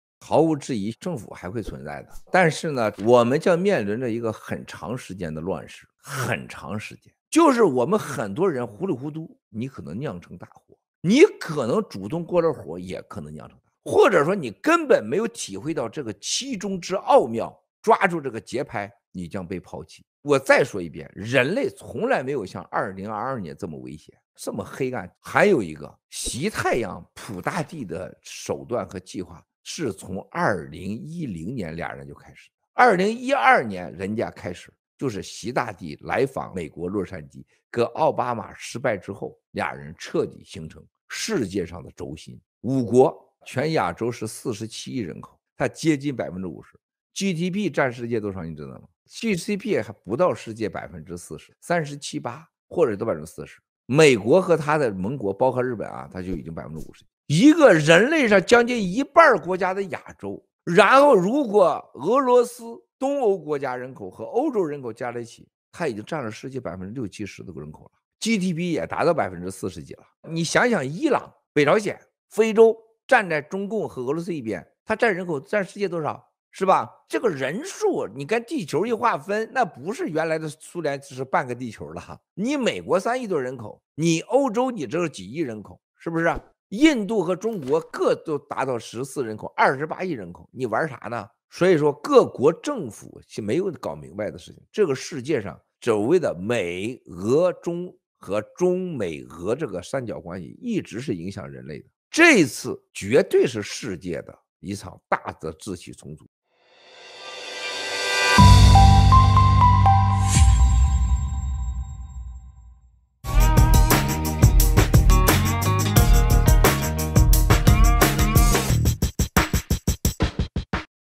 2/25/2022 文贵直播：人类从未像2022年如此黑暗，我们将面临着长期的乱世；习近平和普京从2010年就开始制定邪恶计划；中美俄的三角关系一直在影响着人类，世界秩序将发生大重组！
2/25/2022 Miles Guo: Humanity has never experienced a time as dark as the year of 2022, and we will be living in turmoil for a long time. Xi Jinping and Putin had already started to make their evil plans as early as 2010. The trilateral relationship of the CCP, Russia, and the US has been swaying the course of humanity. The world order will undergo a great restructuring!